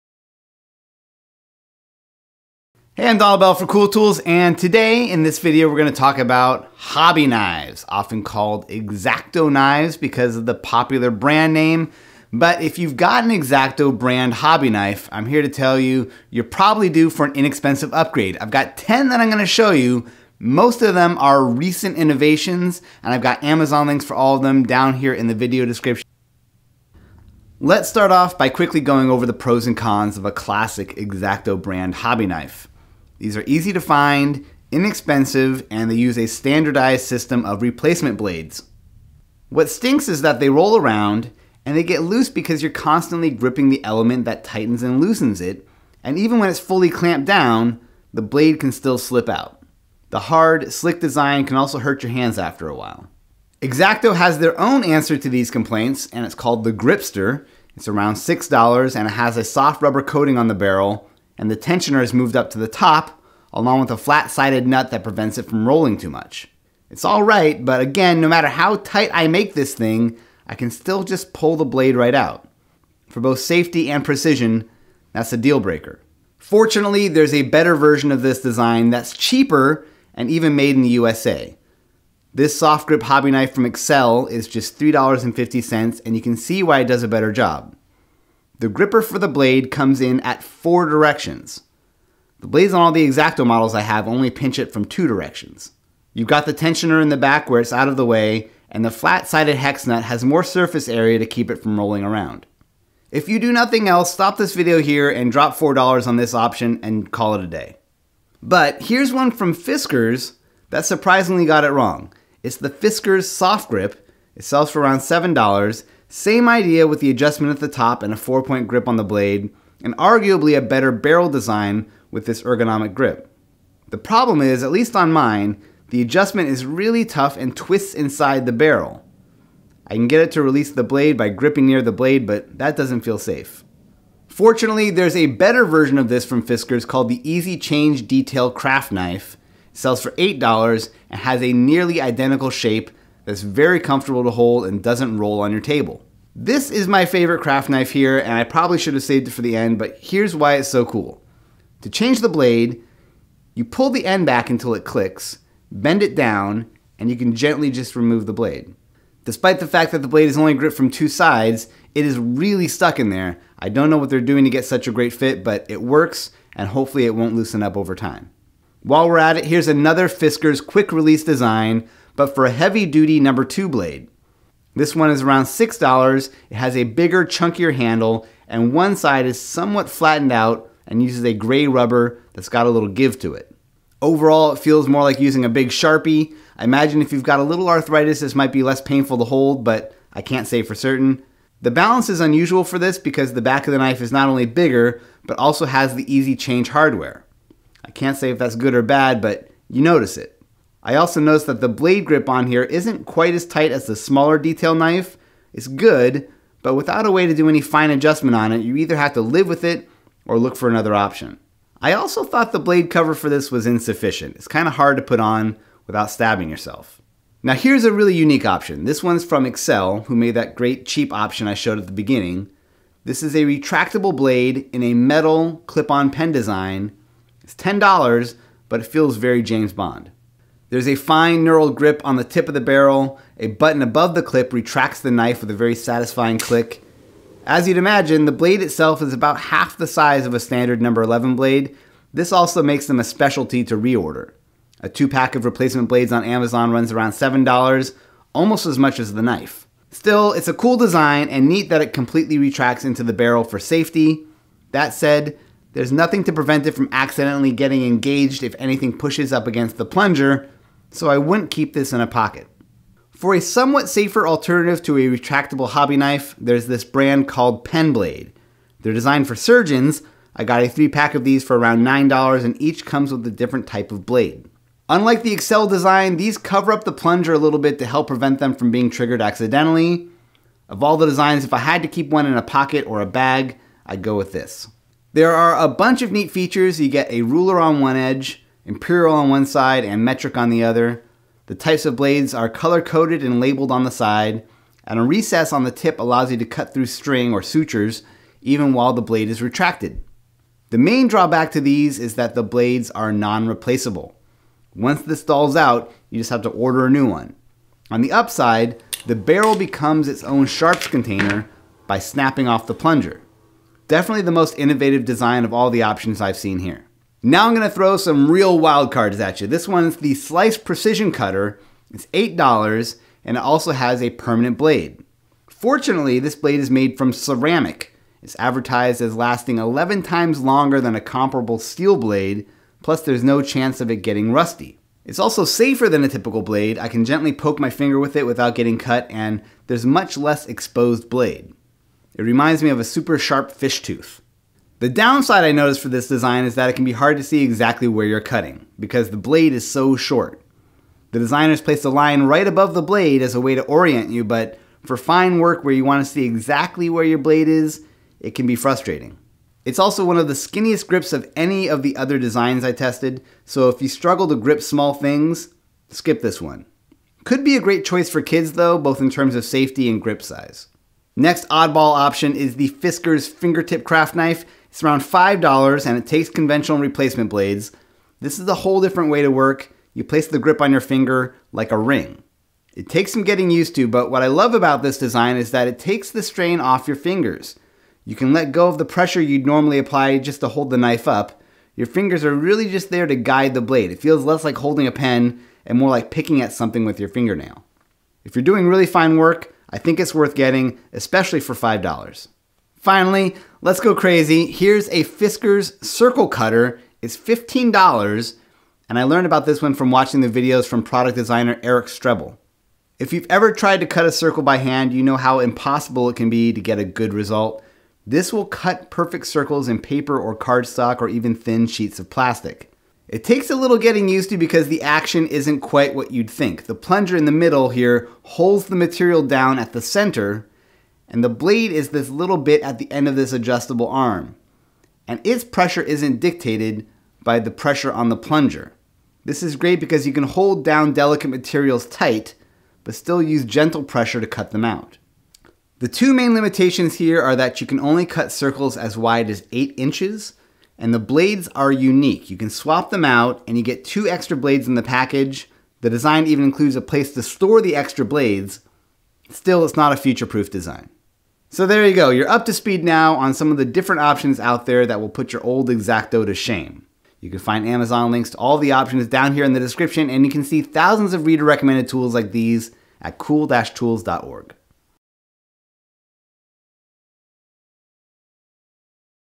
10 Hobby Knife Upgrades
Tools:
X-Acto Gripster Knife,
Soft Grip Hobby Knife,
Fiskars Craft Knife,
Fiskars Easy Change Knife,
Fiskars Heavy-Duty Knife, No. 2 Blade,
Excel Blades K47 Hobby Knife,
Replacement Excel Blades,
Slice Precision Cutter,
Fiskars Fingertip Craft Knife,
Fiskars Circle Cutter,

Transcript:
... Let’s start off by quickly going over the pros and cons of a classic X-Acto brand hobby knife. These are easy to find, inexpensive, and they use a standardized system of replacement blades.
What stinks is that they roll around, they get loose because you’re constantly gripping the element that tightens and loosens it, and even when it’s fully clamped down, the blade can still slip out. The hard, slick design can also hurt your hands after awhile.
X-Acto has their own answer to these complaints. This is called the Gripster, it’s around $6. It has a soft rubber coating on the barrel, and the tensioner is moved to the top, along with a flat-sided nut that prevents it from rolling too much.
It’s alright, but again, no matter how tight I make this thing, I can still just pull the blade right out. For both safety and precision, that’s a deal-breaker.
Fortunately, there’s a better version of this design that’s cheaper and even made in the USA. This soft-grip hobby knife from Excel is just $3.50, and you can see why it does a better job. The gripper for the blade comes at it from four directions. The blades on the X-Acto models I have only squeeze from two directions.
You’ve got the tensioner in the back where it’s out of the way, and the flat-sided hex nut has more surface area to keep it from rolling around.
If you do nothing else, stop this video here, drop $4 on this option and call it a day.
But, here’s one from Fiskars that surprisingly got it wrong. It’s the Fiskars Softgrip. It sells for around $7. Same idea with the adjustment at the top and a four-point grip on the blade. And arguably a better barrel design with this ergonomic rubber grip.
Problem is, on mine at least, the adjustment is really tough and twists inside the barrel. I can get it to release the blade by gripping near the blade, but it doesn’t feel safe.
Fortunately, there’s a better version of this from Fiskars called the Easy Change Detail Craft Knife. It sells for $8 and has a nearly identical shape that’s very comfortable to hold and doesn’t roll on your table.
This is my favorite craft knife. I probably should have saved it for the end. Here’s why it’s so cool. To change the blade, you pull pull the end back until it clicks, bend it down, and you can gently remove the blade.
Despite the fact that the blade is only gripped from two sides, it is really stuck in there. I don’t know what they’re doing to get such a great fit, but it works and hopefully won’t loosen up over time.
While we’re at it, here’s another Fiskars quick release design, but for a heavy duty No 2. Blade. This one is around $6. It has a bigger, chunkier handle, and one side is somewhat flattened out and uses a grey rubber that’s got a little give to it.
Overall, it feels more like using a big Sharpie. I imagine if you’ve got a little arthritis this might be less painful to hold, but I can’t say for certain.
The balance is unusual for this, because the back of the knife is not only bigger, but also has the easy-change hardware. I can’t say that’s good or bad, but you notice it.
I also noticed that the blade grip on here isn’t quite as tight as the smaller detail knife. It’s good, but without a way to do any fine adjustment on it, you either have to live with it or look for another option. I also thought the blade cover for this was insufficient. It’s kinda hard to put on without stabbing yourself.
Now here’s a really unique option. This one’s from Excel, who also made that great, cheap option I showed at the beginning. This is a retractable blade, in a metal, clip-on pen design. It’s $10, but feels very James Bond.
There’s a fine knurled grip on the tip of the barrel. A button above the clip retracts the knife with a very satisfying click. As you’d imagine, the blade itself is about half the size of a standard #11 blade. This also makes them a specialty to reorder. A 2-pack of replacement blades on Amazon runs around $7 -- almost as much as the knife.
Still, it’s a cool design, and neat that it completely retracts into the barrel for safety. That said, there’s nothing to prevent it from accidentally getting engaged if anything pushes up against the plunger. So, I wouldn’t keep this in a pocket.